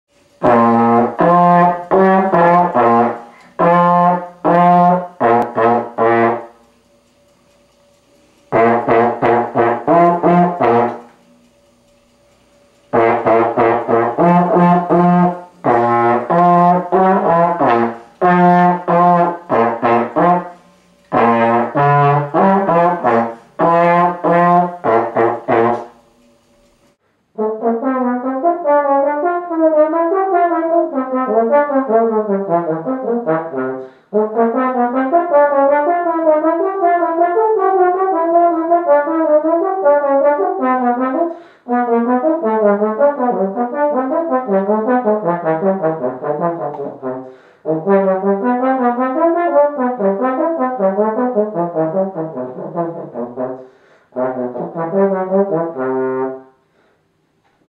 Alex playing trombone at age 8 and 19
Alex is a sophomore in the Crane School of Music at SUNY Potsdam.  Note the same shirt.